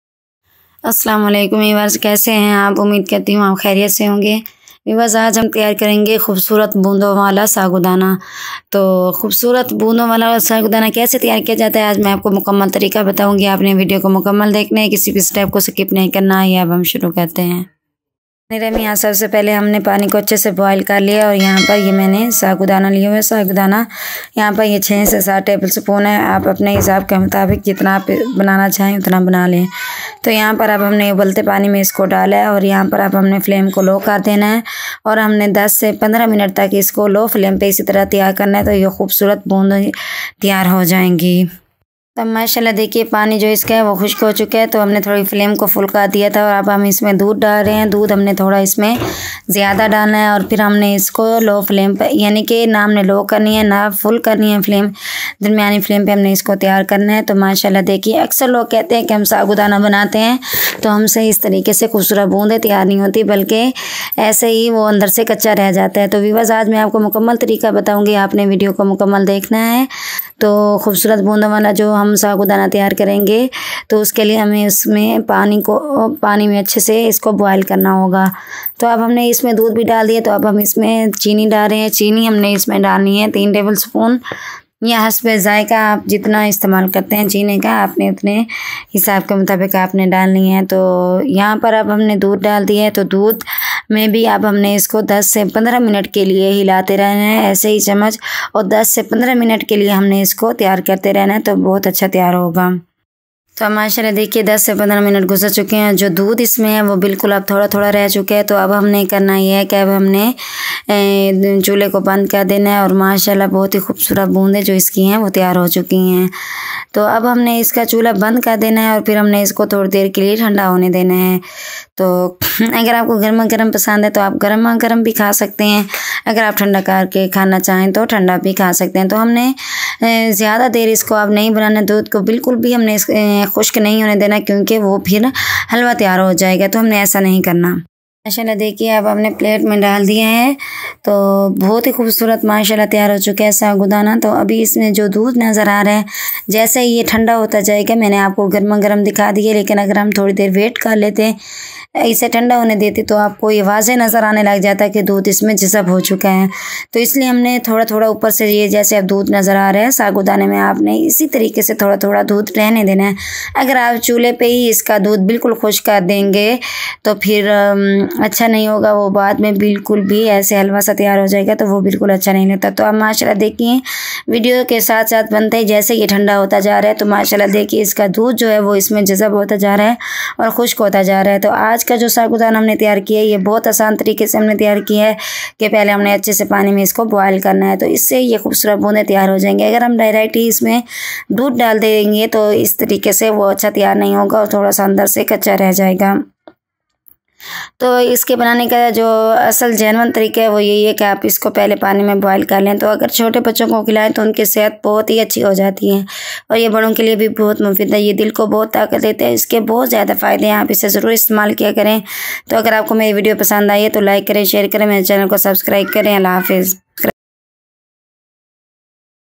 Sago Dana Special at home | ساگو دانہ سپیشل گھر پر بنائیں | Afaf Food Recipe
Today I am going to tell you about a recipe i.e how to cook Sago Dana Special at home | ساگو دانہ سپیشل گھر پر بنائیں | Afaf Food Recipe.